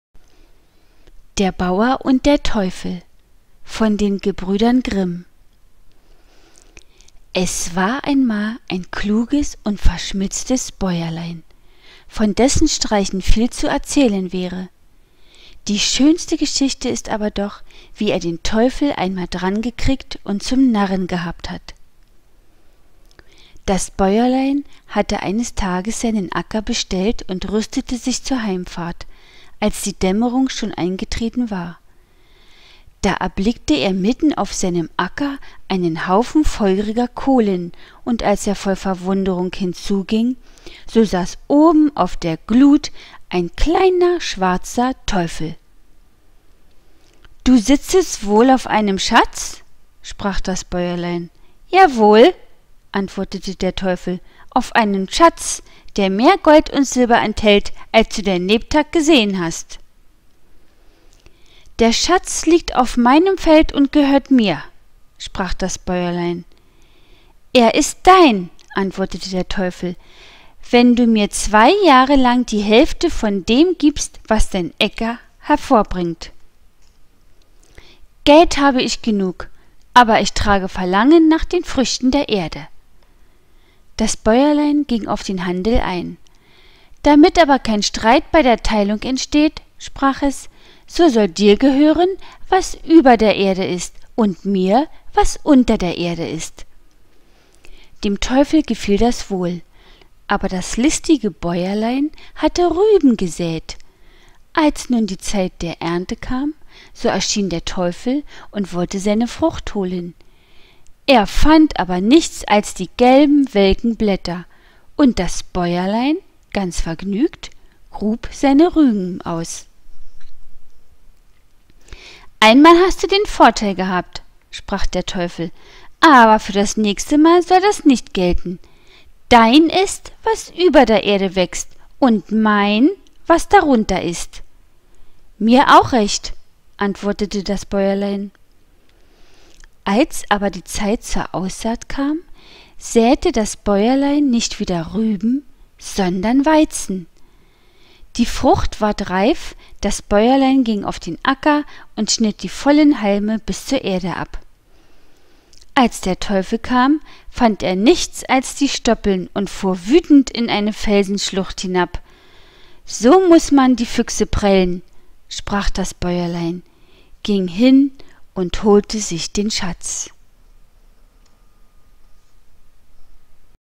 Der Bauer und der Teufel [Gebrüder Grimm]
Buch:       DIE KINDER UND HAUSMÄRCHEN
Autor:       Gebrüder Grimm
Es liest:    Jacqueline Atanasov
Sprache:   Deutsch